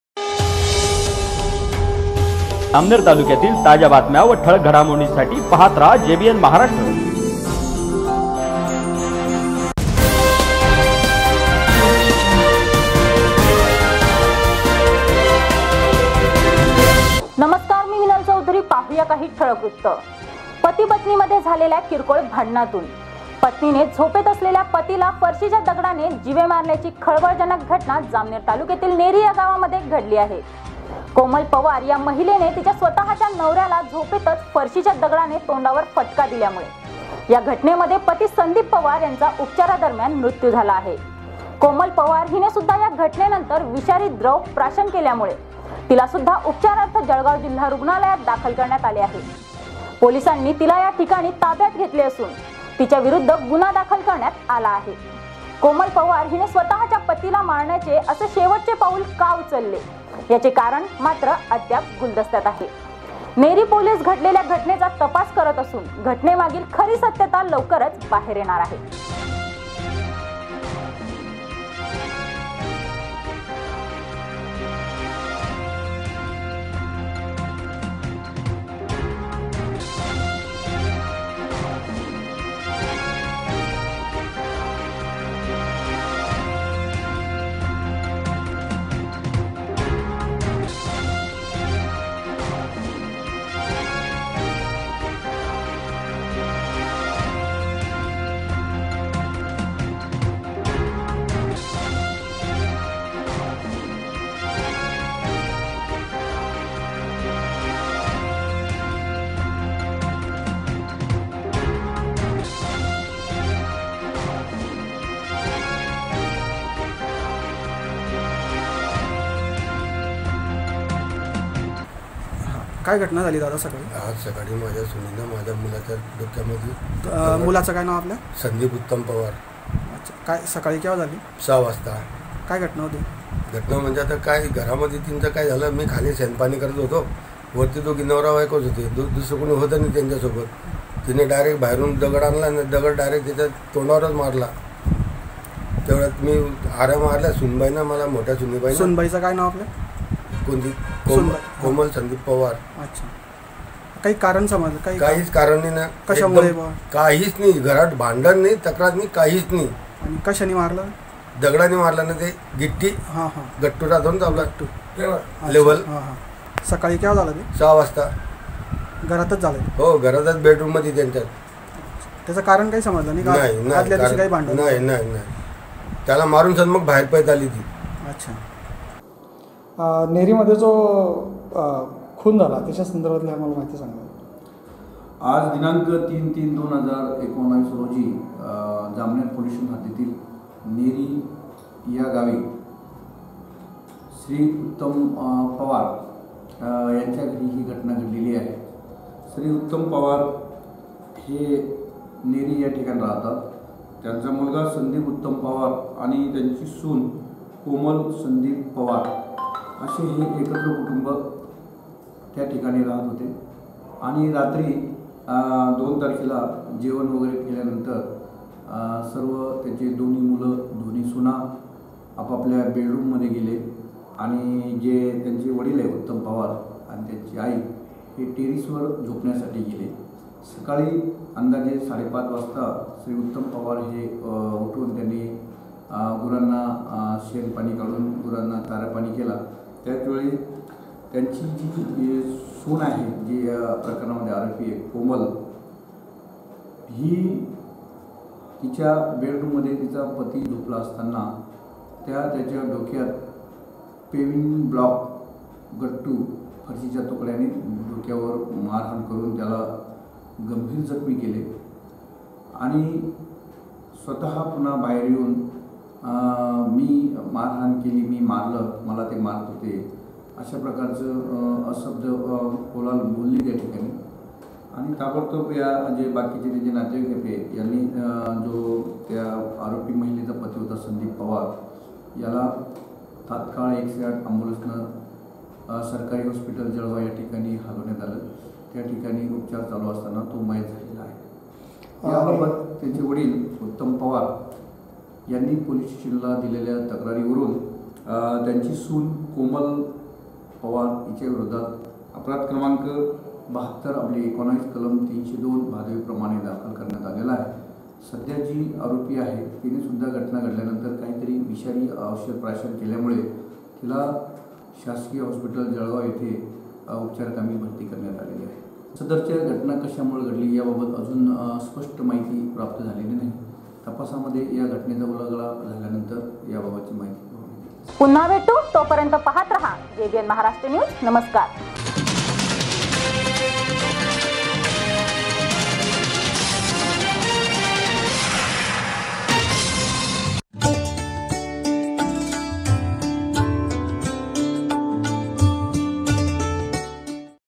jbn maharashtra news jalgaon
नेरी येथे महिलेने केला पतीचा खून.खून करण्या मागचे कारण अजून गुलदस्त्यातच.पोलीस करिता आहे कसून  तपास